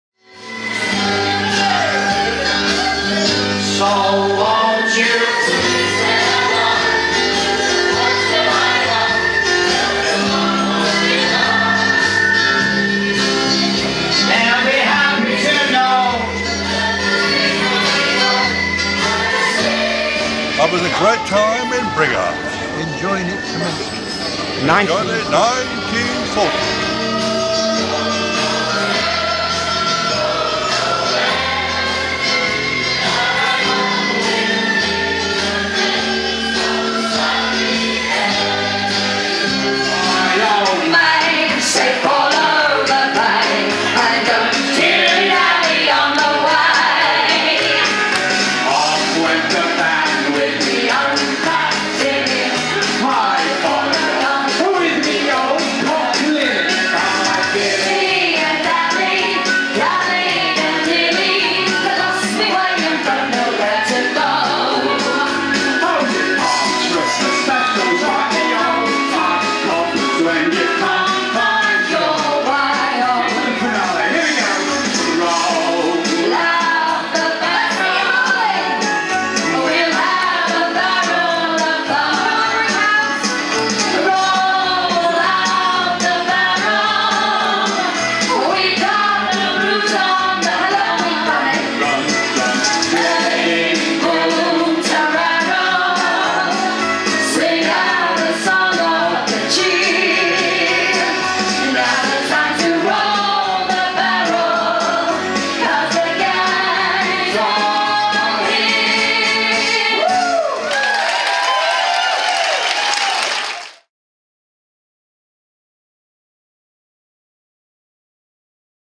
Brighouse 1940s Weekend
The Brighouse 1940s weekend, which takes place on the first weekend in June every year, has become the leading 40s event in West Yorkshire. A free family event for all things vintage. See dancing in the street and hear singers perform all the vintage songs, military and civilian vehicle and military displays. Sit in a replica Spitfire and hear a German tank fire it's gun